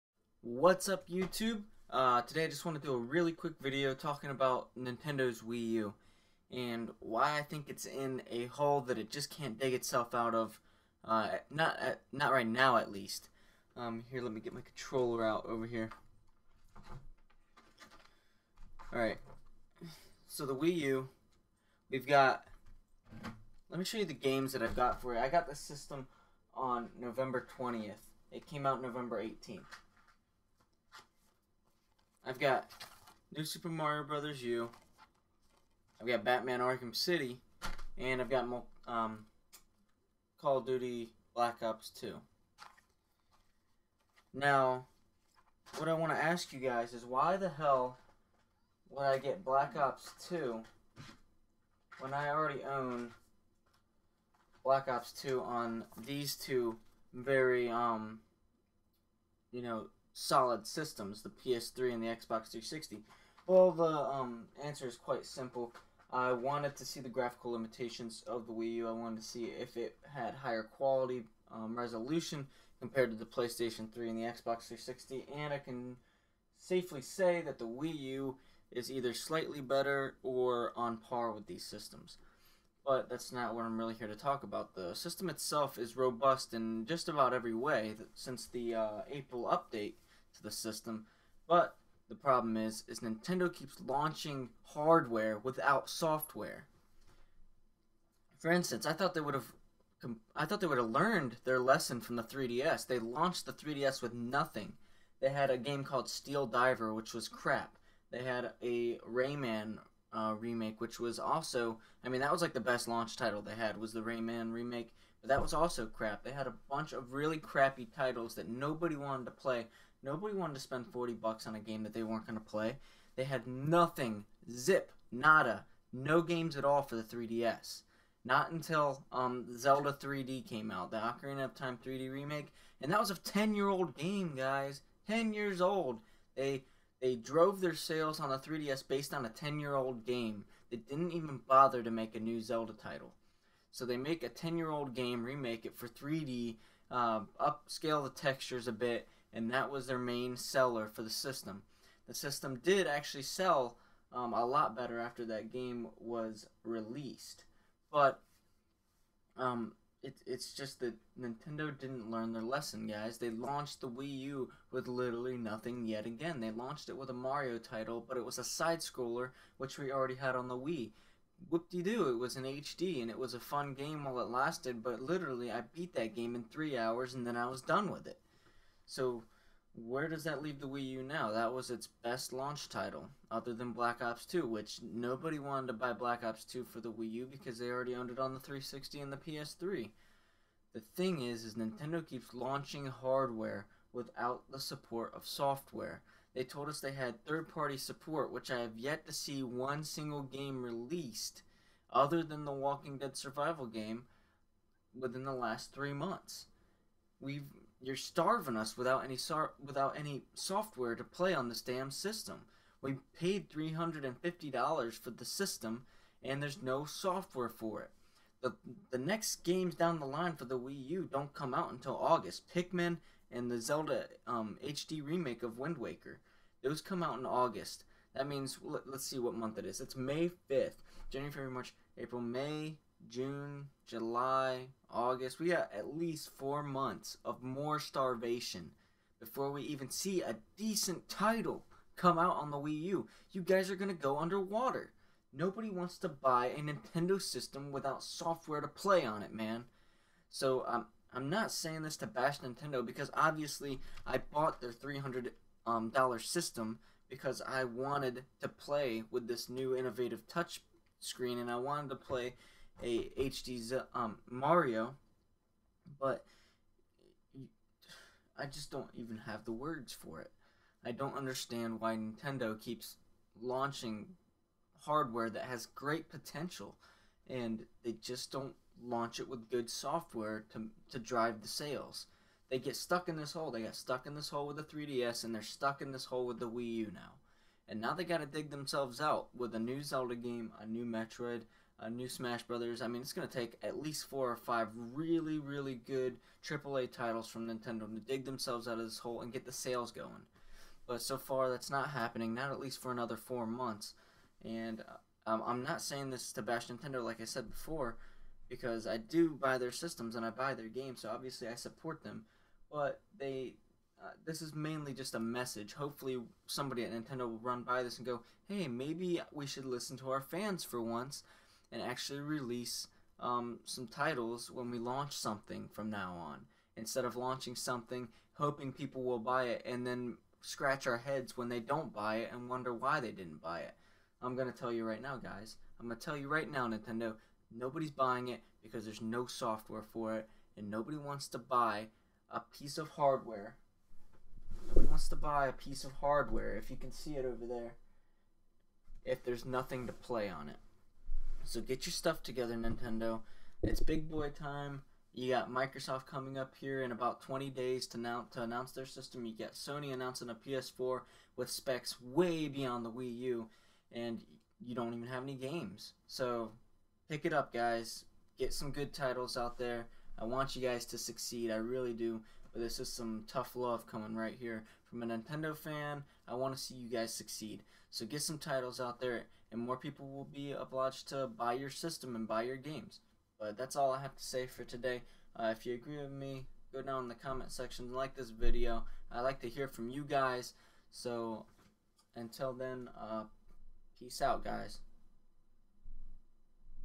Why the Wii U might fail
Nintendo seems to keep launching hardware without software support. Us the consumer must express our opinions to change this pattern.